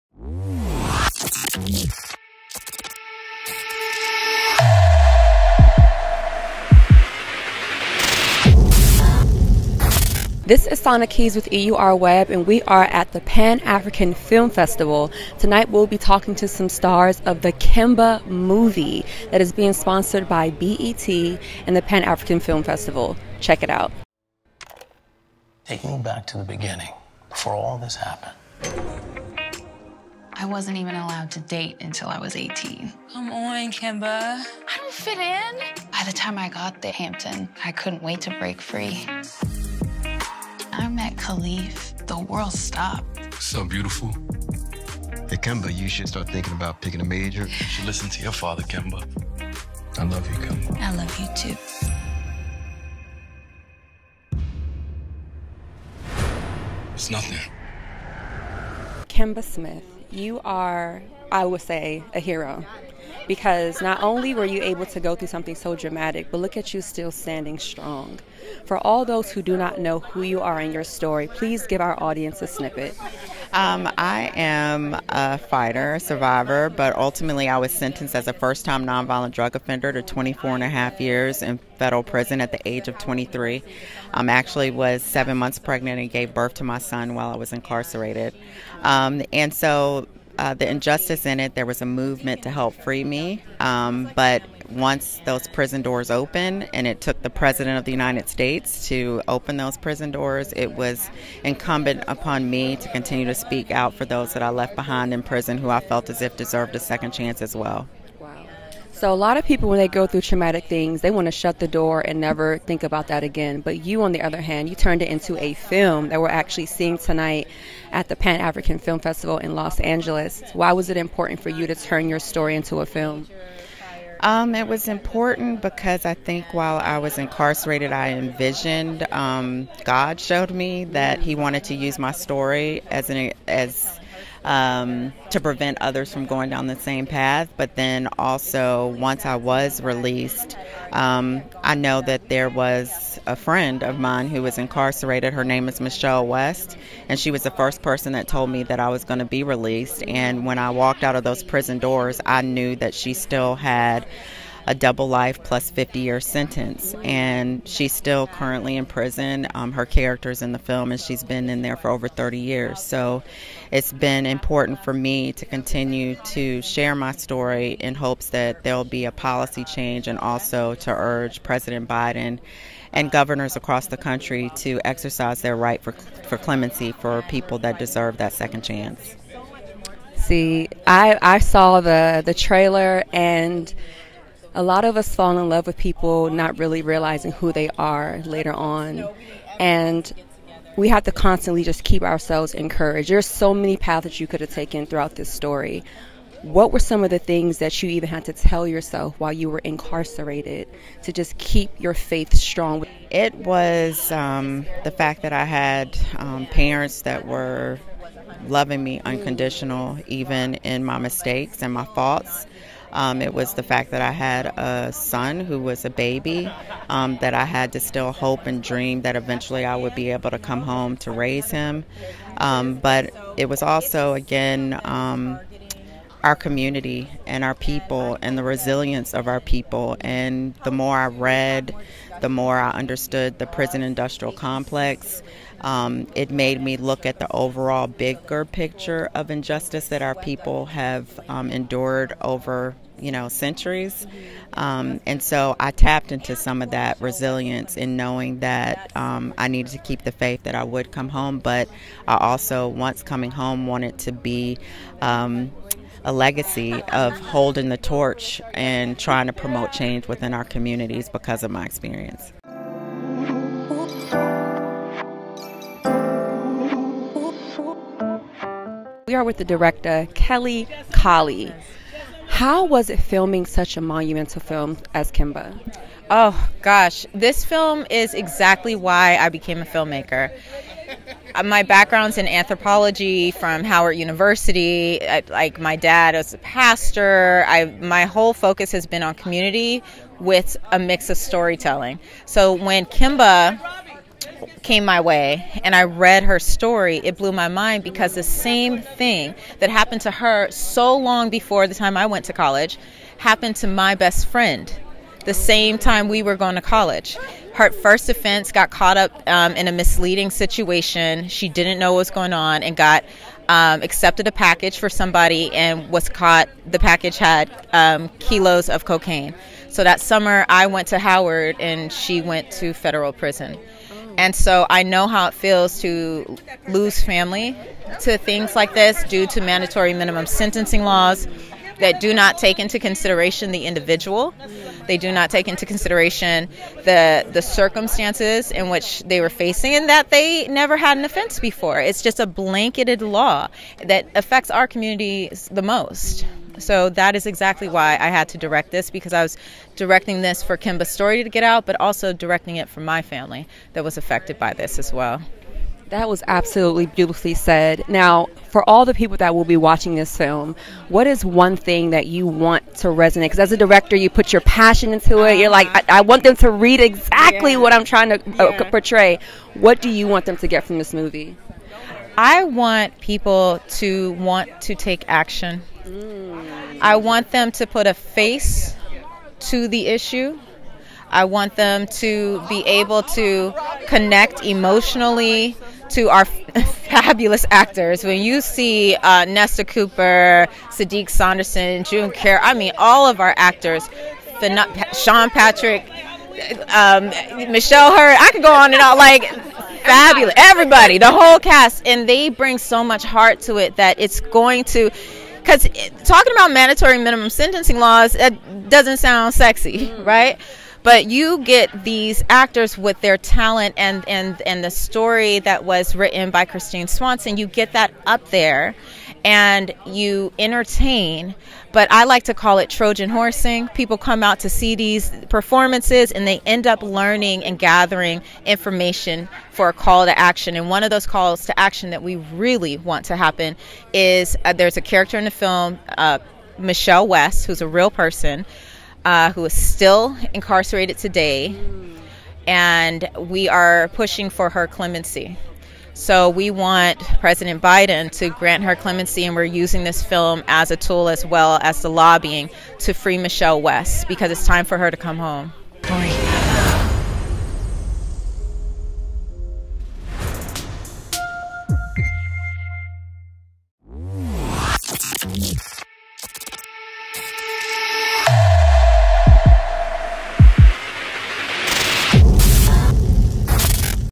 KEMBA Movie | Kemba Smith and Kelley Kali Interviewed by Sana Kibz | BET + PAFF Premiere Los Angeles
Sana Kibz interviews Kemba Smith and Director Kelley Kali at the Los Angeles Premiere of the "Kemba Movie" featured during the 32nd Annual Pan African Film & Arts Festival.

Check out KEMBA on BET Plus and RepresentJustice.org